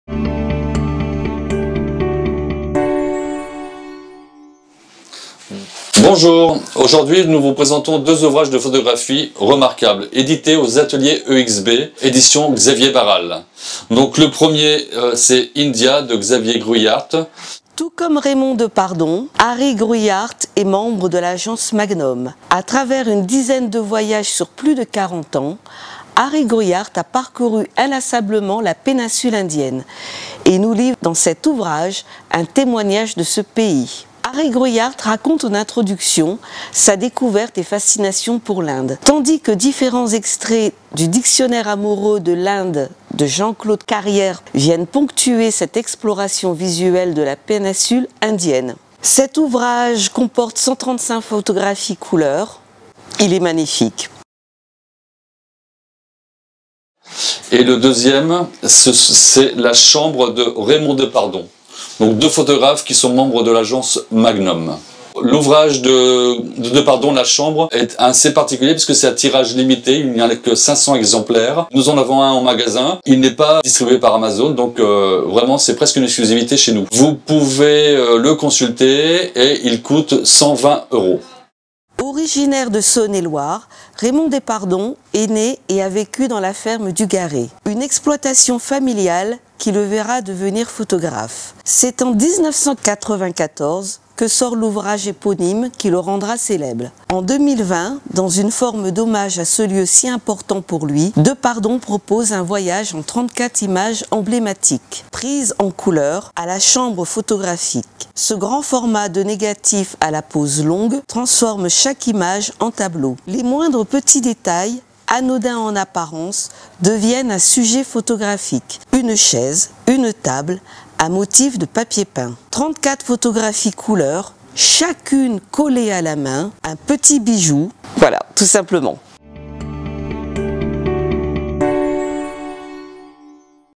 Mevatiana et Alain présentent "India" et "La Chambre" - éditions Atelier EXB
Mevatiana et Alain s'associent pour vous présenter deux livres Photo exceptionnels aux éditions Atelier EXB !

⏩ LA CHAMBRE de Raymond DEPARDON (500 exemplaires seulement)

Vous pouvez nous joindre au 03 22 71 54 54 pour plus d'informations et de conseils !